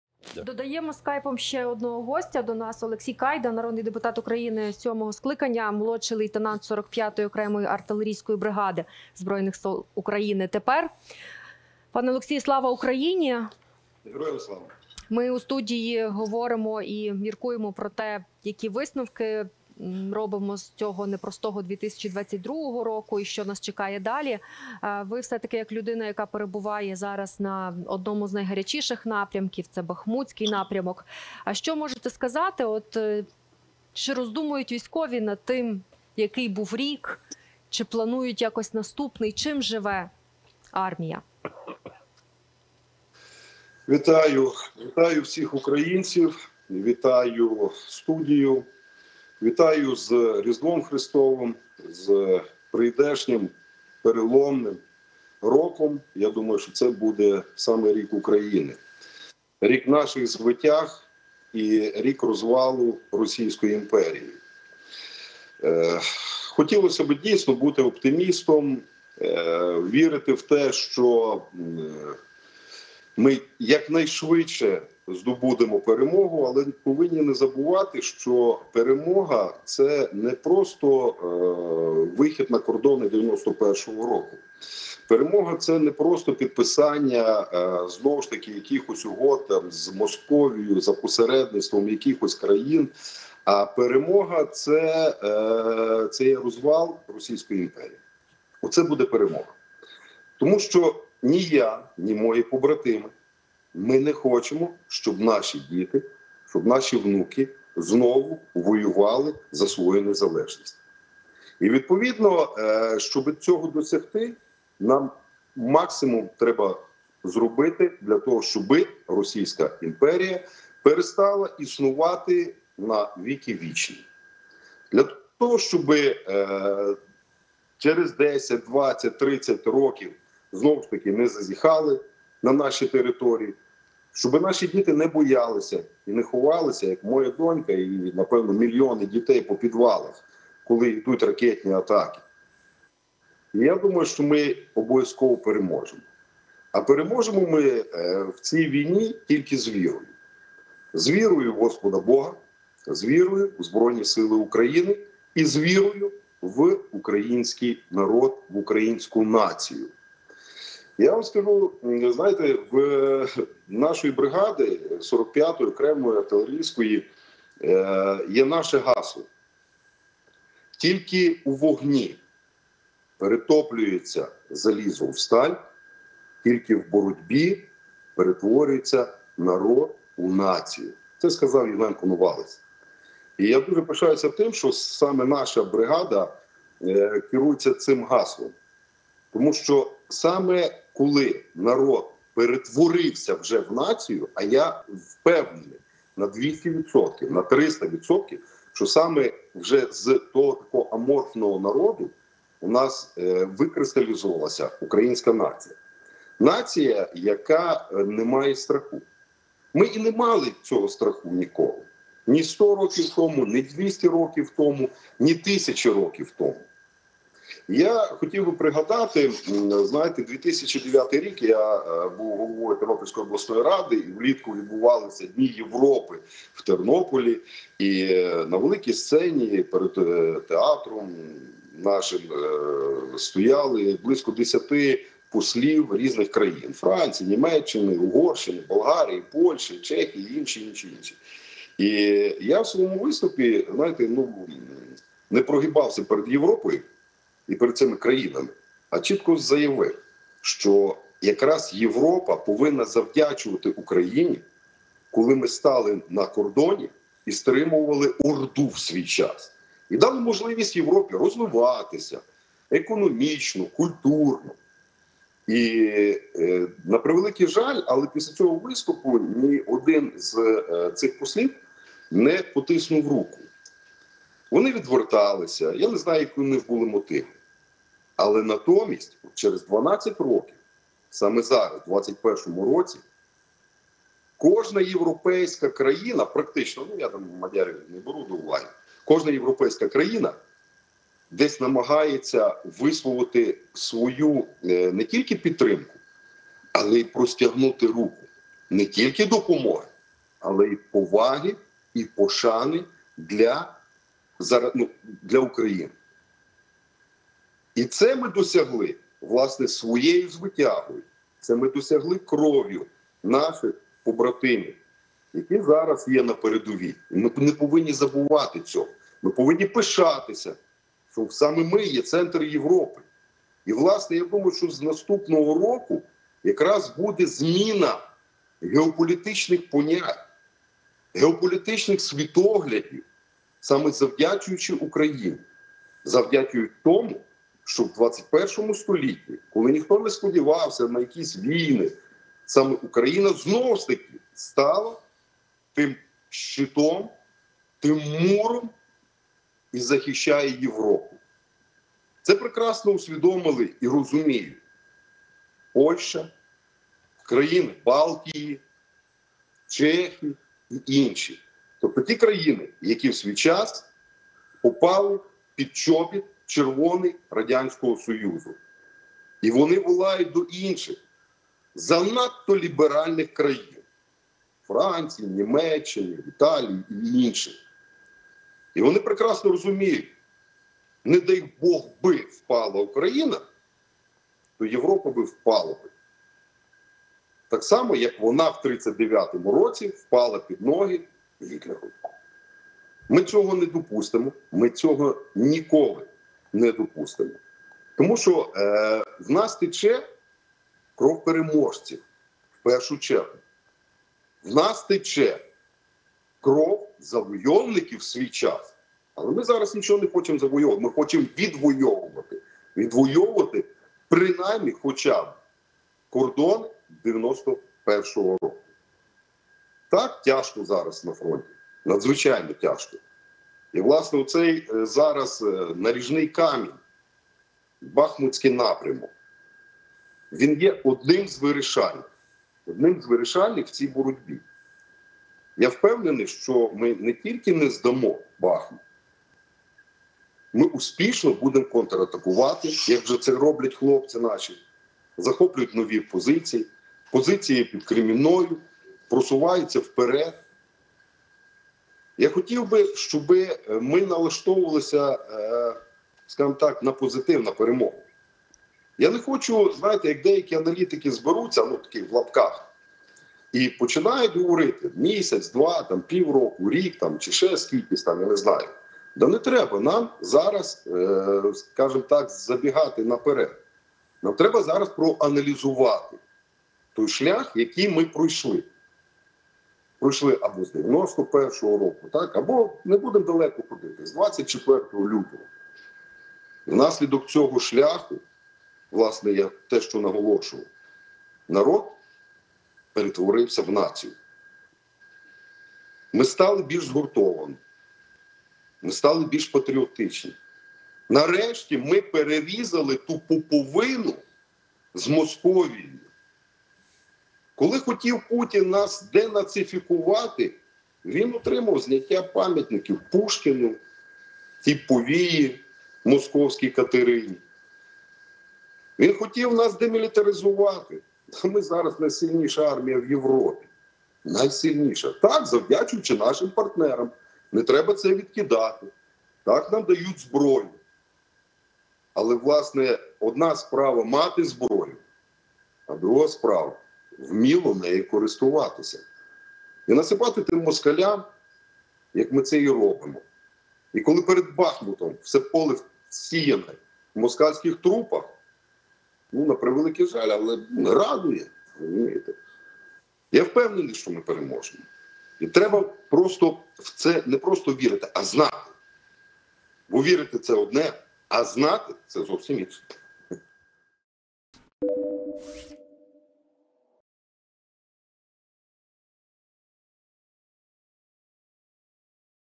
«Якби Україна впала - впала б Європа» | Воїн 45-ої бригади, нардеп КАЙДА
Перемога - це розпад РФ. Ми не хочемо, аби наші діти і внуки знову воювати за свою незалежність”, - Олексій Кайда, народний депутат України VII скликання, молодший лейтенант 45-ої окремої артилерійської бригади ЗСУ